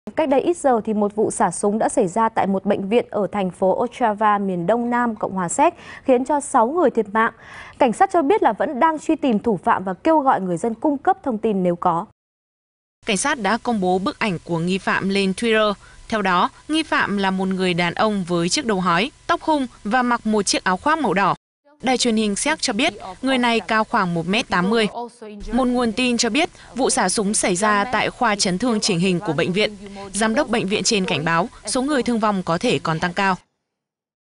Xả súng tại bệnh viện ở Séc
Cách đây ít giờ, một vụ xả súng đã xảy ra tại một bệnh viện ở thành phố Ostrava, miền Đông Nam Cộng hòa Séc, khiến 6 người thiệt mạng. Cảnh sát cho biết vẫn đang truy tìm thủ phạm và kêu gọi người dân cung cấp thông tin nếu có.